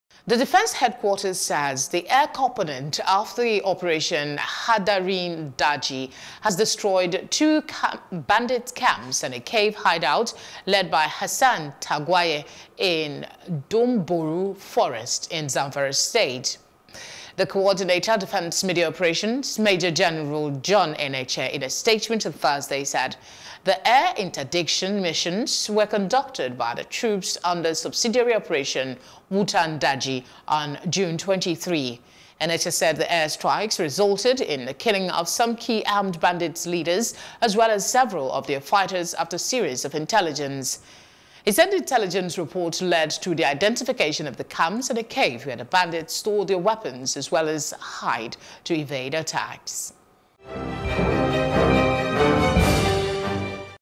Scores Killed In Air Raid On Doumbourou Forest
The Defence Headquarters says the air component of Operation Hadarin Daji has destroyed two bandits’ camps and a cave hideout led by Hassan Tagwaye in Doumborou Forest, Zamfara.

The Coordinator, Defence Media Operations, Maj.-Gen. John Enenche, in a statement on Thursday, said the air interdiction missions were conducted by the troops under the subsidiary Operation Wutan Daji on June 23.